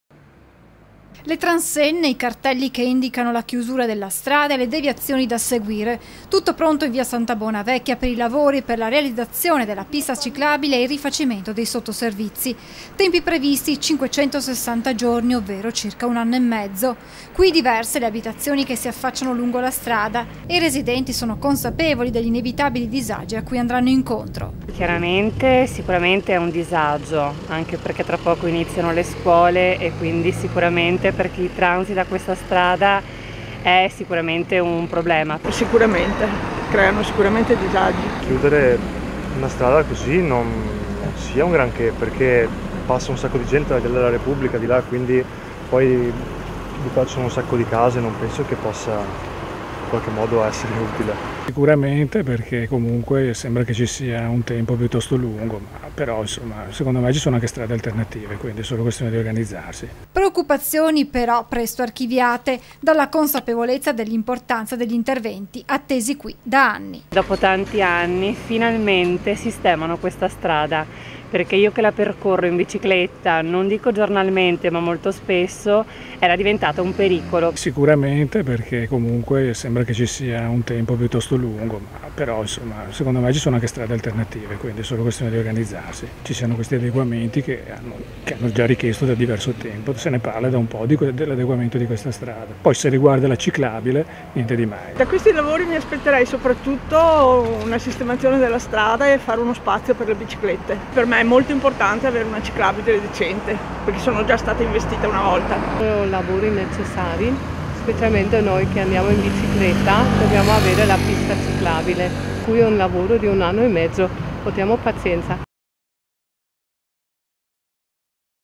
TG TREVISO (28/08/2018) - S.BONA VECCHIA CHIUSA PER OLTRE UN ANNO:«PORTIAMO PAZIENZA, LAVORI NECESS
TG TREVISO (martedì 28 agosto 2018) - Le transenne, i cartelli che indicano la chiusura della strada e le deviazioni da seguire. Tutto pronto in Via Santa Bona vecchia per i lavori per la realizzazione della pista ciclabile e il rifacimento dei sottoservizi. Tempi previsti: 560 giorni ovvero circa un anno e mezzo. Qui diverse le abitazioni che si affacciano lungo la strada, e i residenti sono consapevoli degli inevitabili disagi a cui andranno incontro.
Preoccupazioni presto archiviate però dalla consapevolezza dell'importanza degli interventi attesi da anni.